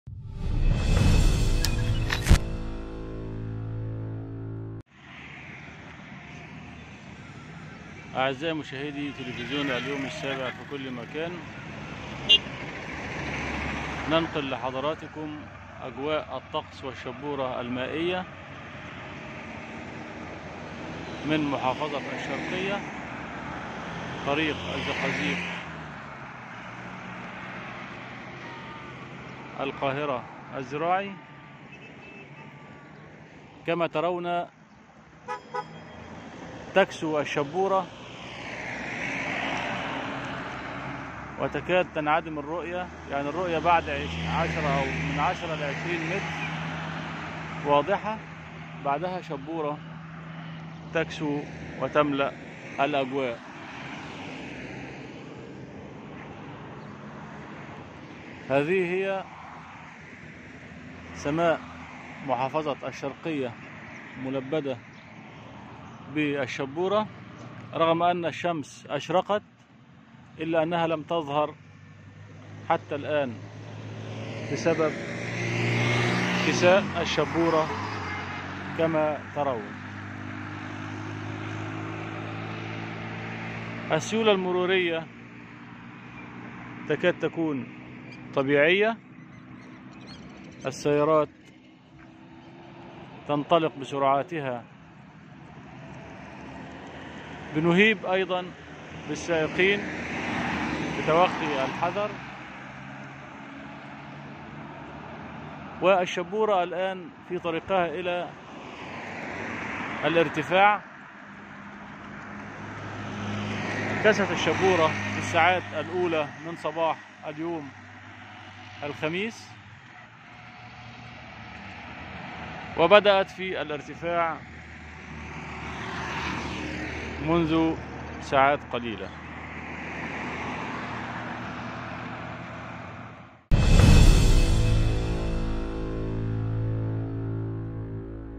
بث مباشر.. سيولة مرورية وشبورة خفيفة على طريق الزقازيق
اليوتيوب
تويتر